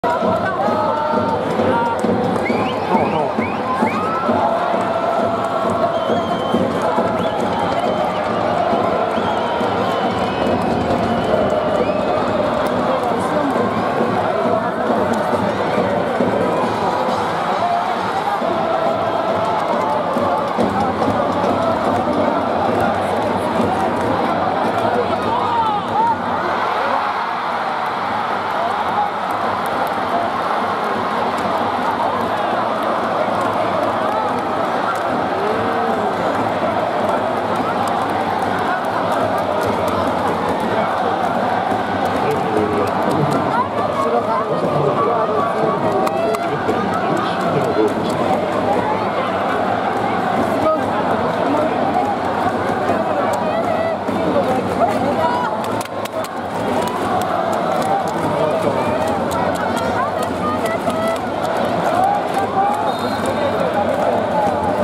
エウシーニョに決められ サガン鳥栖 2-2
後半13分、ボールを左サイドで縦に個ばれ、最後はグラウンダーのセンタリングを中でエウシーニョに合わされ、サガン鳥栖 2-2 川崎フロンターレ。

J1　第18節　サガン鳥栖 2-3 川崎フロンターレ 。
2017年7月8日(土) 20:18～20:19頃
ベストアメニティスタジアムのメインスタンド2階席より撮影